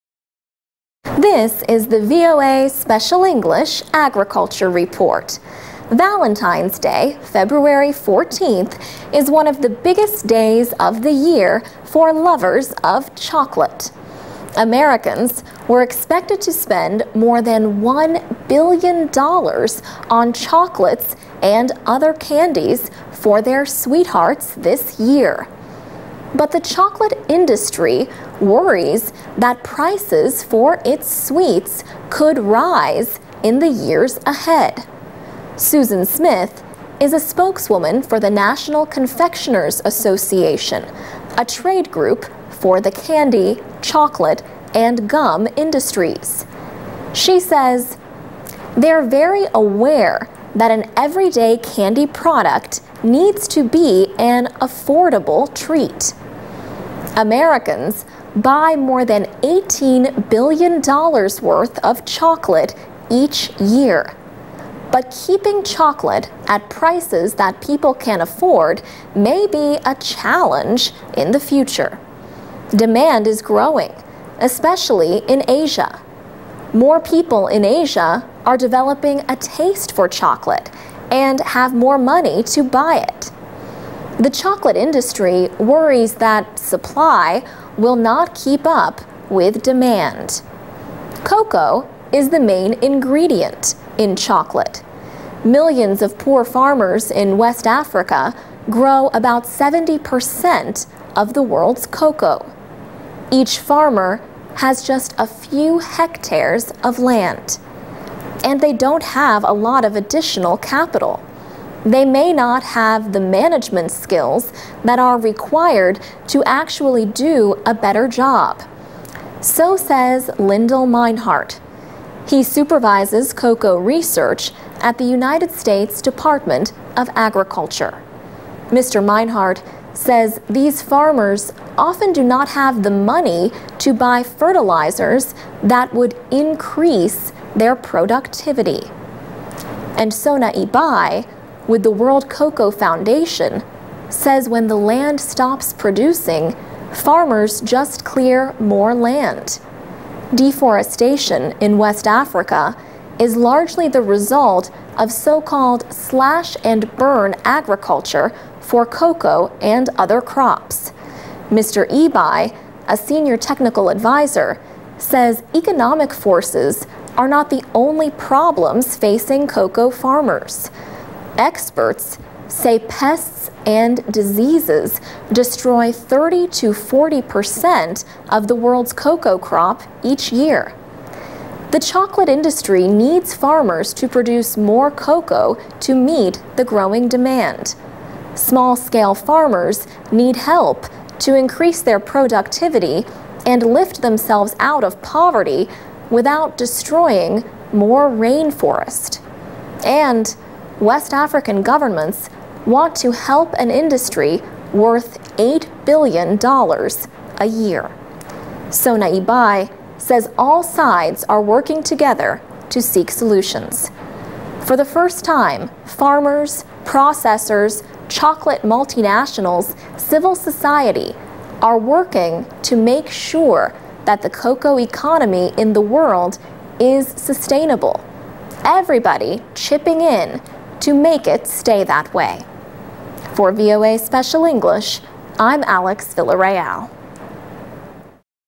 Will Cost of Cocoa Raise the Price of Valentine's Day?
Valentine's Day, February fourteenth, is one of the biggest days of the year for lovers of chocolate. Americans were expected to spend more than one billion dollars on chocolates and other candies for their sweethearts this year. But the chocolate industry worries that prices for its sweets could rise in the years ahead. Susan Smith is a spokeswoman for the National Confectioners Association, a trade group for the candy, chocolate and gum industries. She says, "They're very aware that an everyday candy product needs to be an affordable treat." Americans buy more than eighteen billion dollars worth of chocolate each year. But keeping chocolate at prices that people can afford may be a challenge in the future. Demand is growing, especially in Asia. More people in Asia are developing a taste for chocolate, and have more money to buy it. The chocolate industry worries that supply will not keep up with demand. Cocoa is the main ingredient in chocolate. Millions of poor farmers in West Africa grow about seventy percent of the world's cocoa. Each farmer has just a few hectares of land. "And they don't have a lot of additional capital. They may not have the management skills that are required to actually do a better job." So says Lyndel Meinhardt. He supervises cocoa research at the United States Department of Agriculture. Mr. Meinhardt says these farmers often do not have the money to buy fertilizers that would increase their productivity. And Sona Ebai with the World Cocoa Foundation says when the land stops producing, farmers just clear more land. Deforestation in West Africa is largely the result of so-called slash-and-burn agriculture for cocoa and other crops. Mr. Ebai, a senior technical adviser, says economic forces are not the only problems facing cocoa farmers. Experts say pests and diseases destroy thirty to forty percent of the world's cocoa crop each year. The chocolate industry needs farmers to produce more cocoa to meet the growing demand. Small-scale farmers need help to increase their productivity and lift themselves out of poverty, without destroying more rainforest. And West African governments want to help an industry worth eight billion dollars a year. Sona Ebai says all sides are working together to seek solutions. "For the first time, farmers, processors, chocolate multinationals, civil society are working to make sure that the cocoa economy in the world is sustainable -- everybody chipping in to make it stay that way." For VOA Special English, I'm Alex Villarreal.(Adapted from a radio program broadcast 14Feb2012)